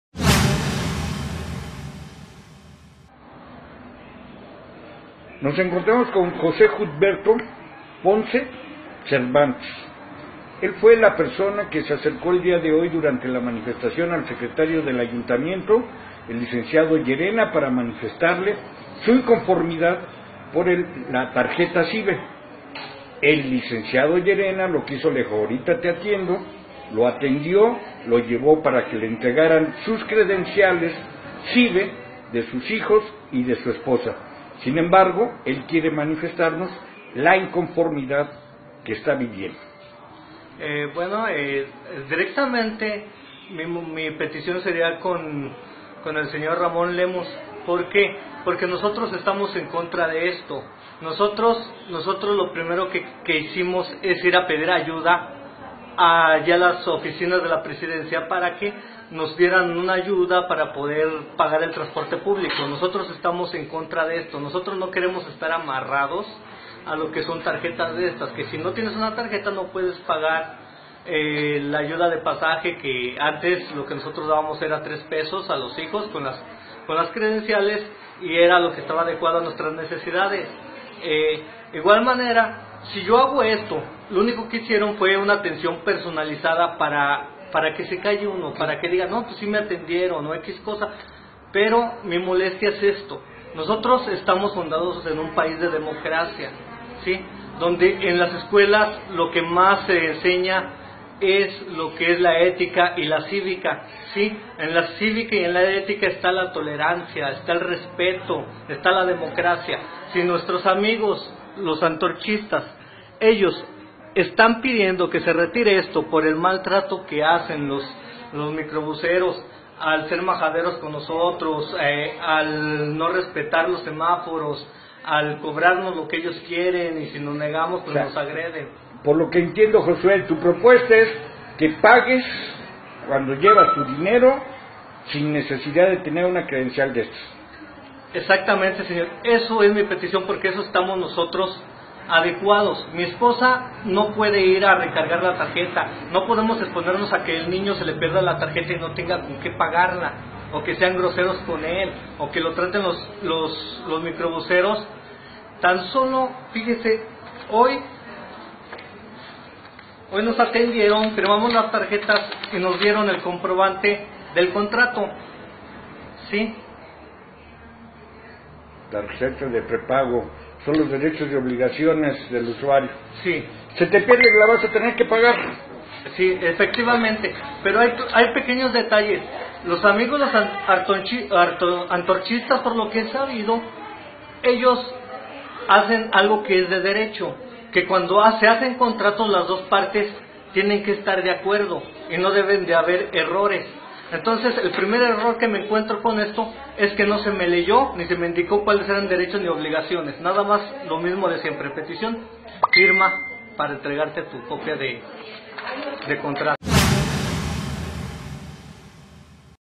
VALE SABER/ LE QUISIERON CALLAR REGALÁNDOLE SUS TARJETAS SIBE
Josue Cutberto Ponce encaró hoy al Secretario del Ayuntamiento, durante la manifestación de inconformidad por el pago del transporte, le llaman "modernización", sin ser del grupo de Antorcha...El señor Yerena se lo quiso quitar de encima, le dijo que le atendería personalmente, y para que se callara tramitó las tarjetas preferentes de sus hijos y esposa... y ese no era el asunto